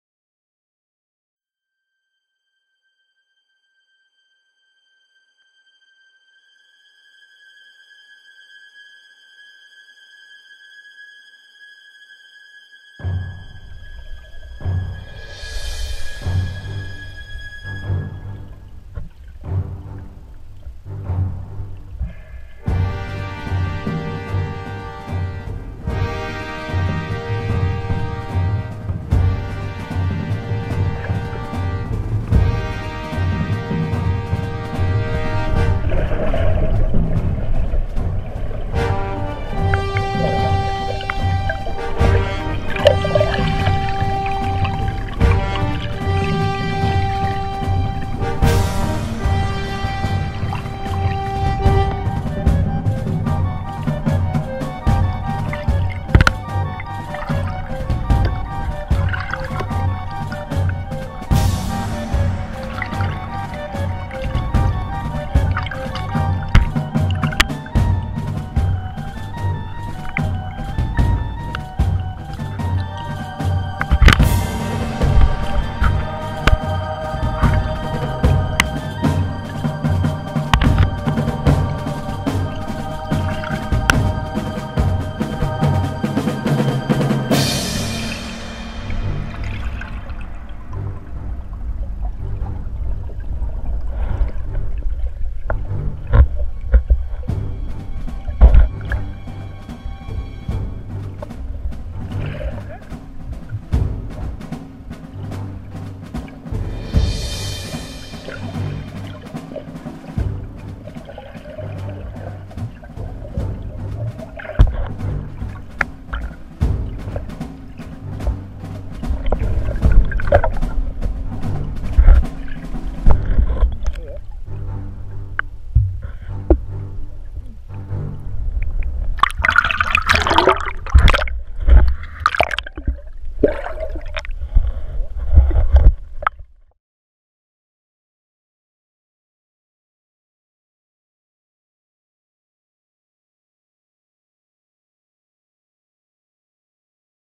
Collision with Rocky Protrusion whilst Freediving!
Whilst I was getting footage for my 'Freshwater Freediving'  Video, I had the misfortune to accidentally hit my head on a rocky outcrop that protruded from the side of the dive site. As a result we had to cut short our videoing and visit the local hospital so I could get stitched up! I actually was recording at the time and from the angle of the camera pointing upwards, it showed what happened (although I didn't see it with the mask I was wearing at the time - I actually avoided one rocky outcrop but in the process hit my head on the one coming out from the other side). I left this footage out of the original video but months later decided that I would put a short video together featuring that particular dive.